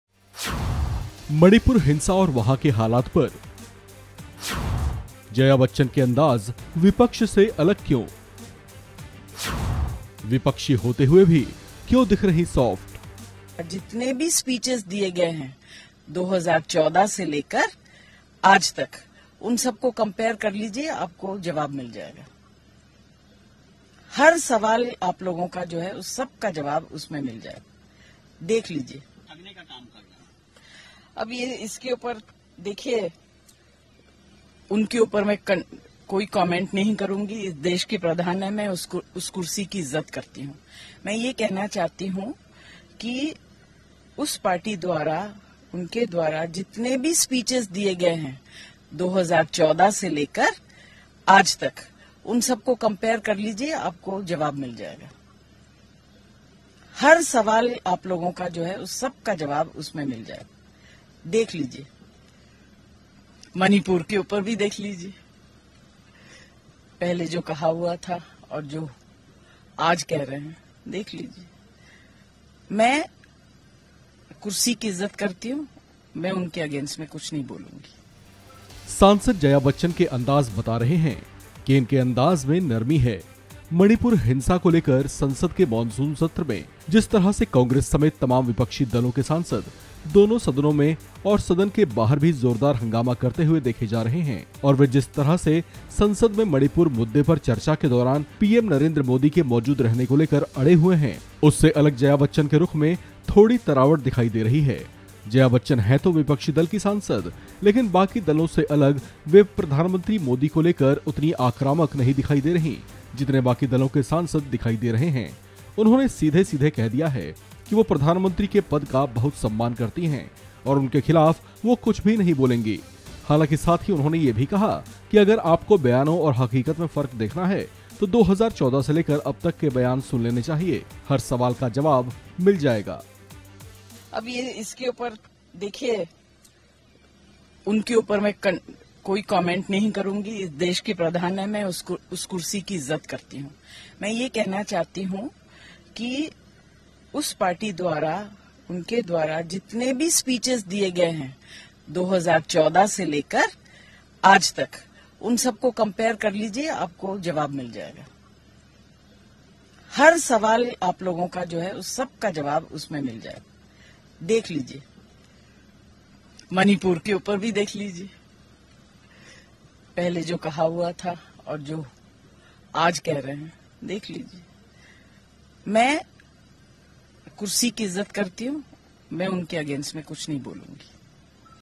PM Narendra Modi को Jaya Bachchan ने Manipur Violence के लिए क्या सुनाया ? | BJP | वनइंडिया हिंदी
Manipur Women Video: सांसद जया बच्चन (Jaya Bachchan) के अंदाज़ बता रहे हैं, कि इनके अंदाज़ में नरमी है। मणिपुर हिंसा (Manipur Violence) को लेकर संसद के मॉनसून सत्र (Monsoon Session) में जिस तरह से कांग्रेस (Congress) समेत तमाम विपक्षी दलों (Opposition Parties) के सांसद (Parliament), दोनों सदनों में और सदन के बाहर भी ज़ोरदार हंगामा करते हुए देखे जा रहे हैं। और वे जिस तरह से संसद में मणिपुर मुद्दे (Manipur Issue) पर चर्चा के दौरान पीएम नरेंद्र मोदी (PM Narendra Modi) के मौजूद रहने को लेकर अड़े हुए हैं, उससे अलग जया बच्चन के रुख में थोड़ी तरावट दिखाई दे रही है। (Manipur) (Violence) (Manipur women case) (Manipur Women Video) (Manipur Viral Video)